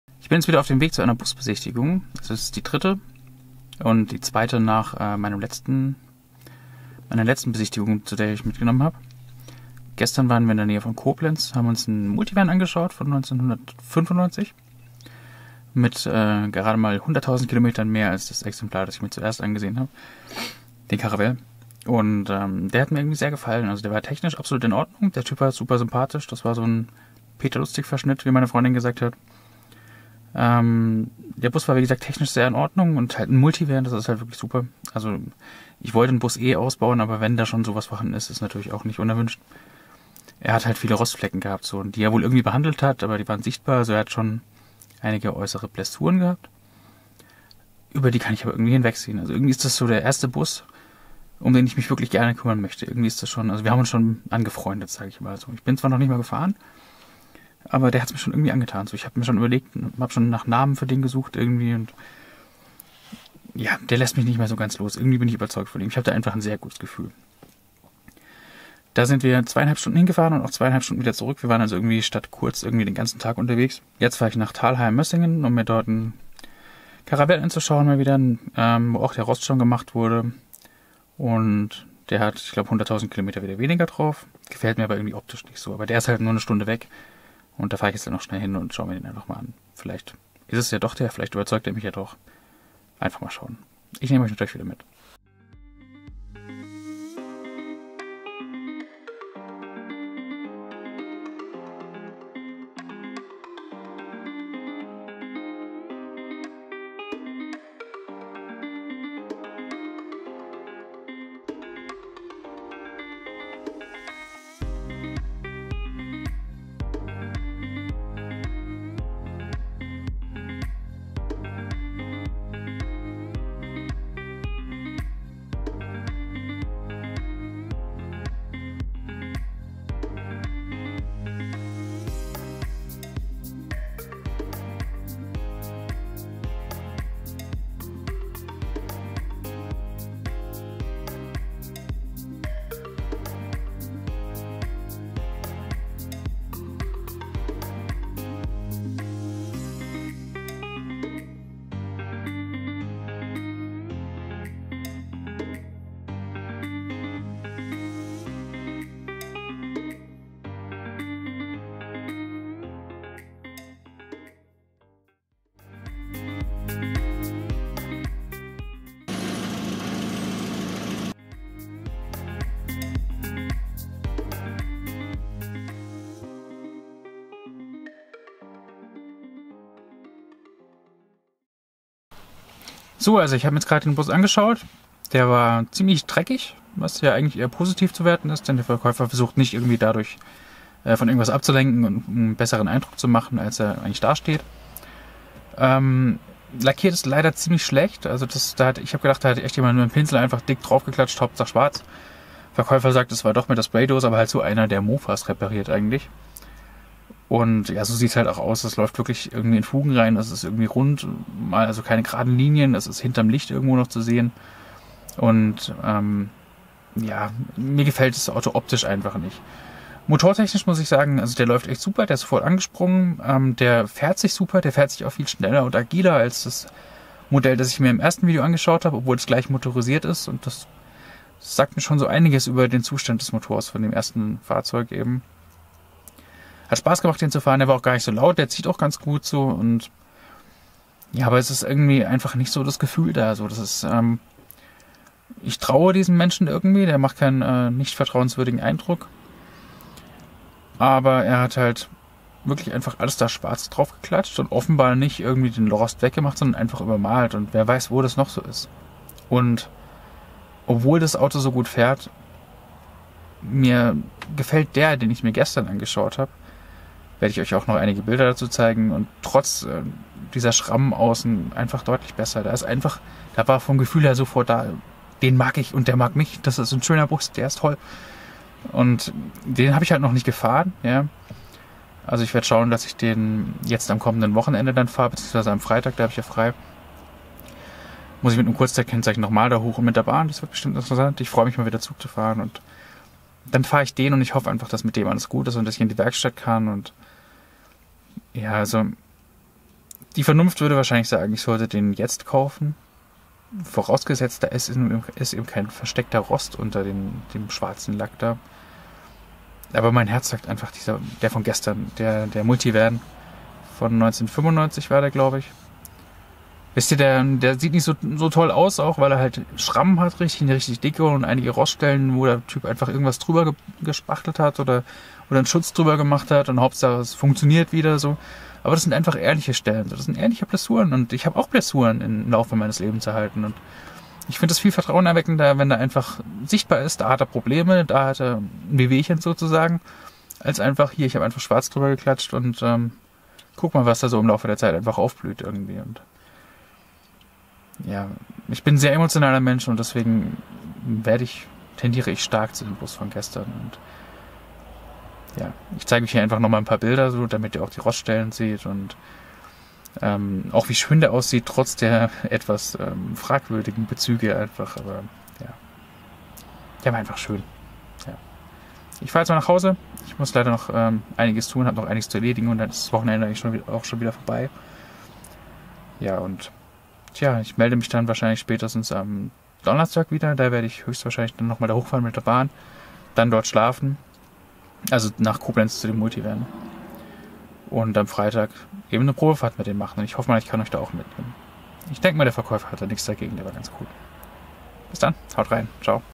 VW T4 Caravelle Besichtigung - Wird es dieser Camper für uns ? / Vegan aber nett
Ich bin weiter auf der Suche nach einem geeigneten VW-Bus und schaue mir in diesem Video, nach einem Multivan, einen weiteren T4 Caravelle an. Ob es dieses mal der richtige ist?

Unsere Video Ausstattung:

Lumix FZ300
Go Pro Hero 8
iPhone 12
Zoom H1n
Rode SmartLav+
Adapter TRRS auf TRS
Passende Softbox
Yeti X